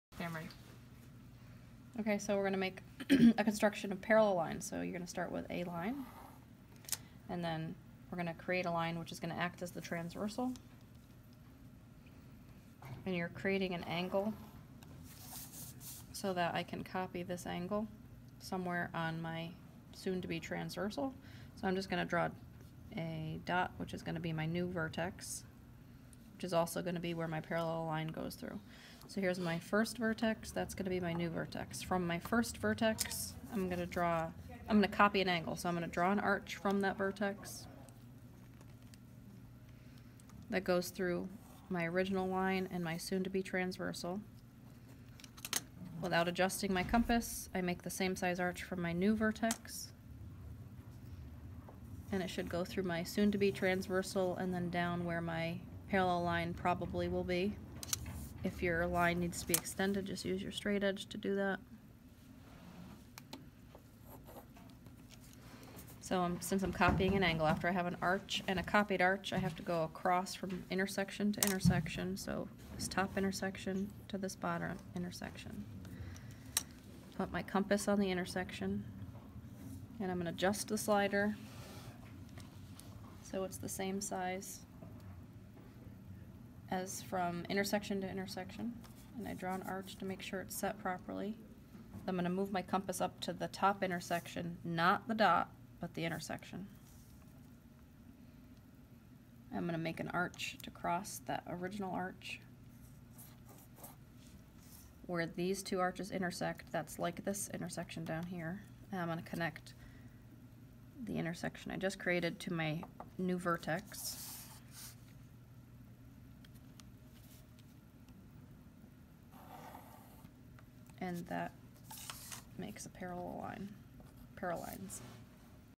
Geometric Constructions: Parallel Lines
Use a compass and a straightedge too construct parallel lines (with copying an angle)